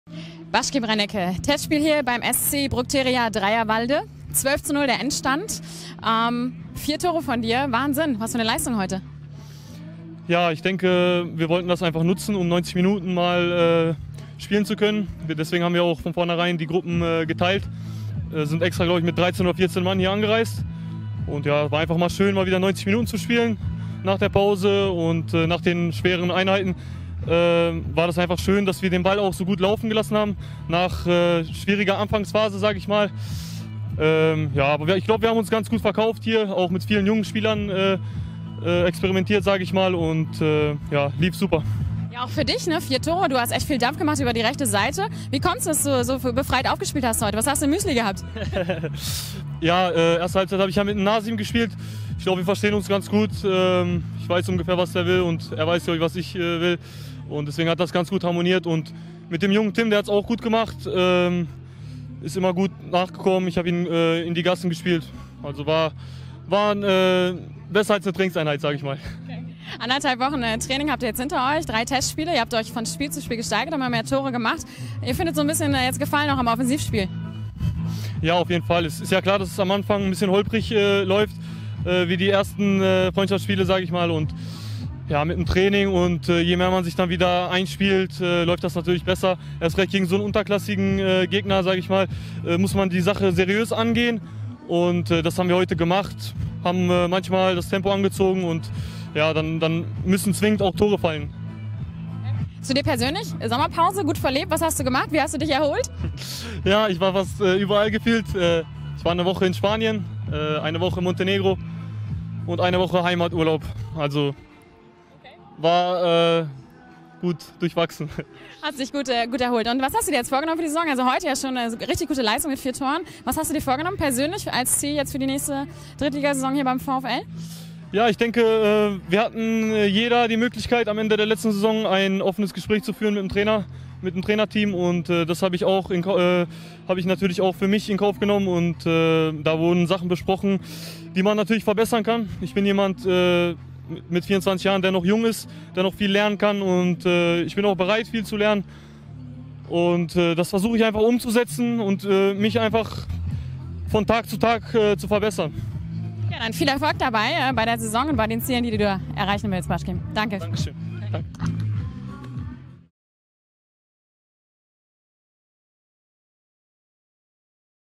VfL-TV | Renneke trifft vier Mal in Dreierwalde
Bashkim Renneke war beim Testspiel beim SV Brukteria Dreierwalde mit vier Toren und einer engagierten Vorstellung der "Man of the match". VfL-TV hat sich nach dem Spiel mit ihm unterhalten.